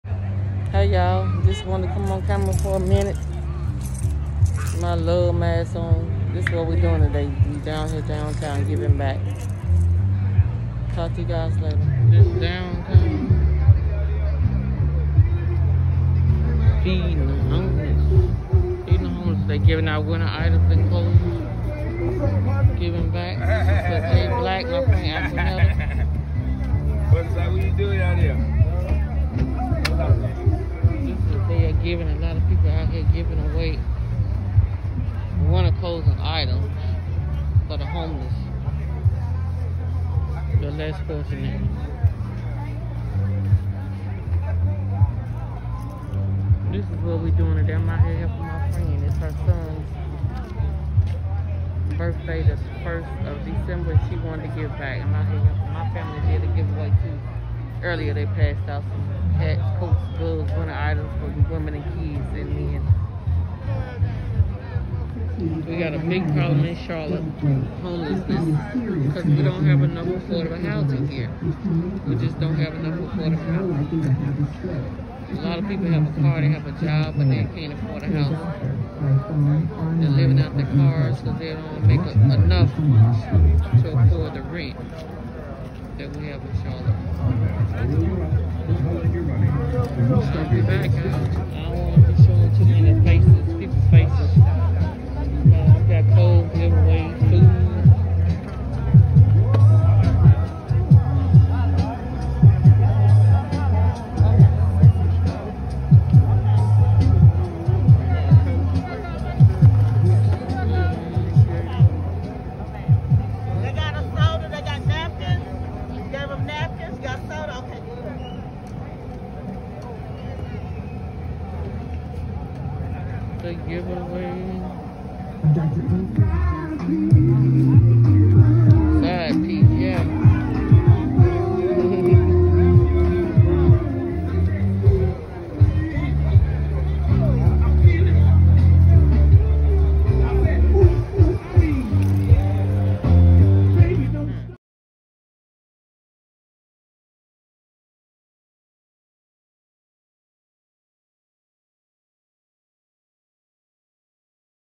HOMELESSNESS IS BAD HERE||SUPER SATURDAY||GIVING BACK
SUPPORTING ANOTHER MOTHER WHO LOST HER SON BLESS THE HOMELESS! GIVING BACK MAKES ME FEEL MORE BLESSED